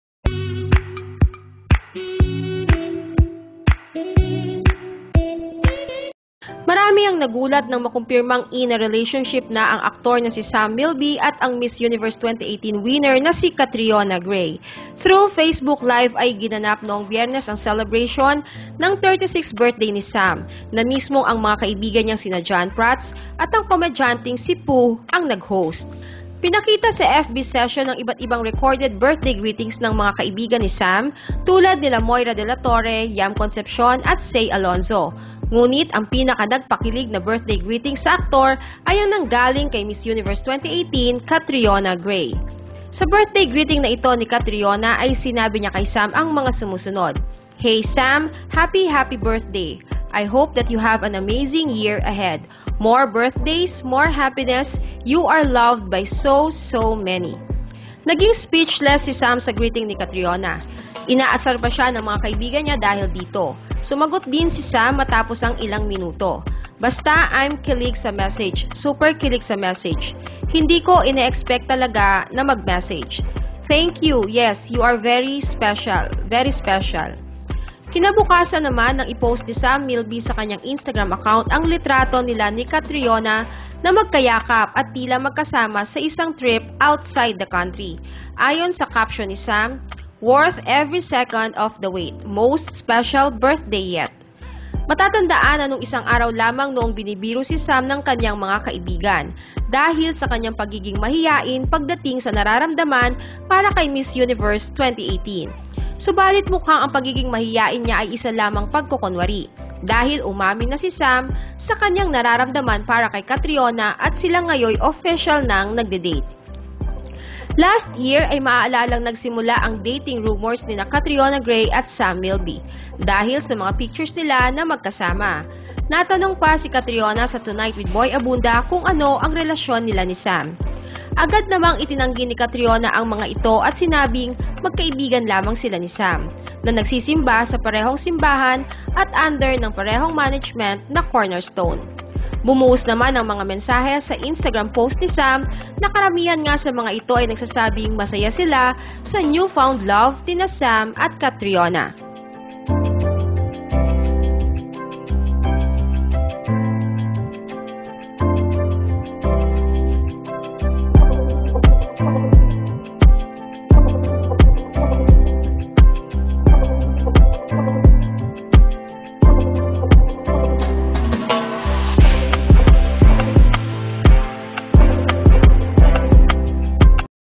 Watch! Nakakakilig na PAG-AMIN ni Sam Milby at Catriona Gray sa RELATIONSHIP Nila!
Marami ang nagulat nang makumpirmang in a relationship na ang actor na si Sam Milby at ang Miss Universe 2018 winner na si Catriona Gray. Wath the video to know more.

Sam Milby Catrion Gray relationship